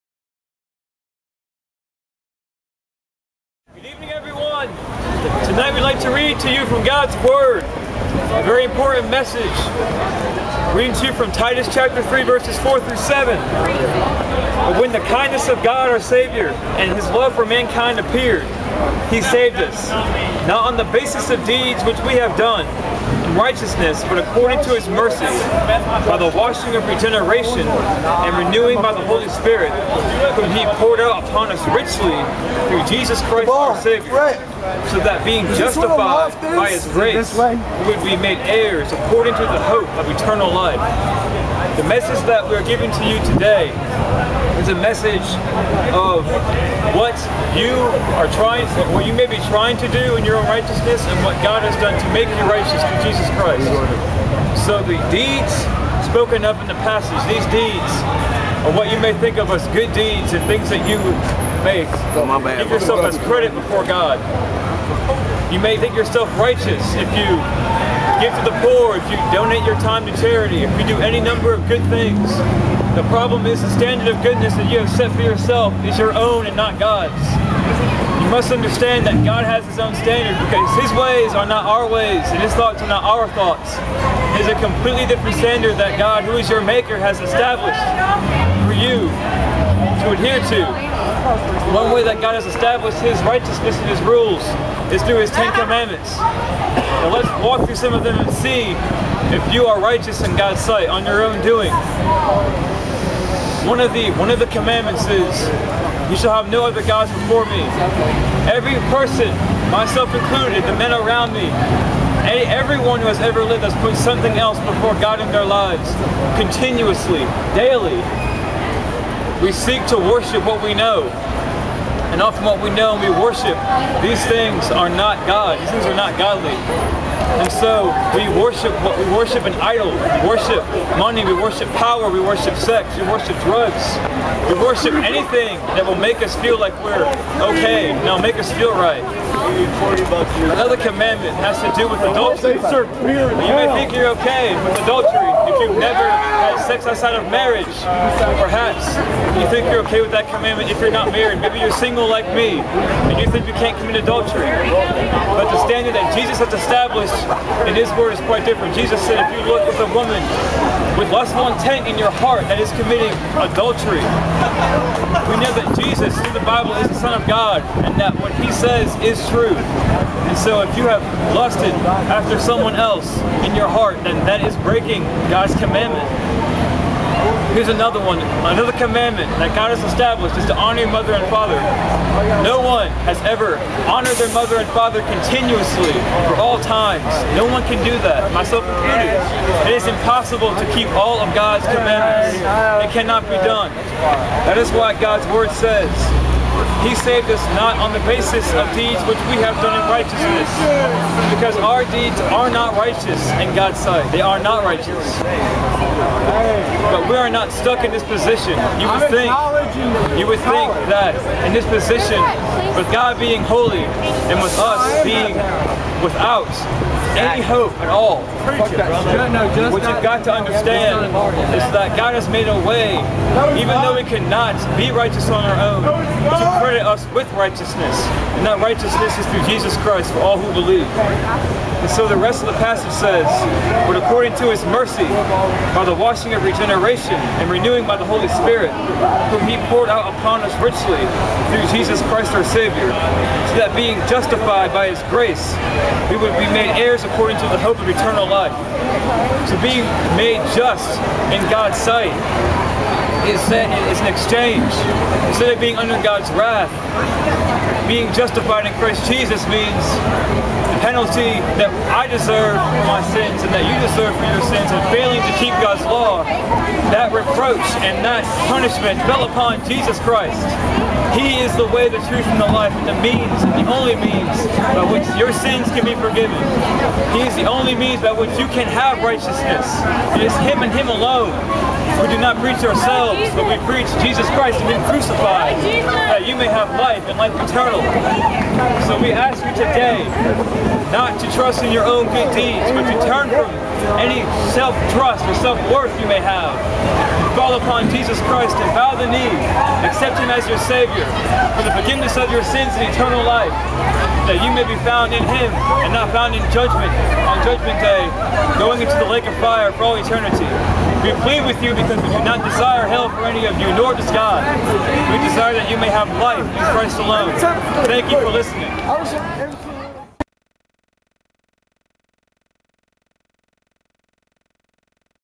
Preach On! Athens (Alex Nunez)
Alex Nunez proclaiming the law and the Gospel in Athens, GA.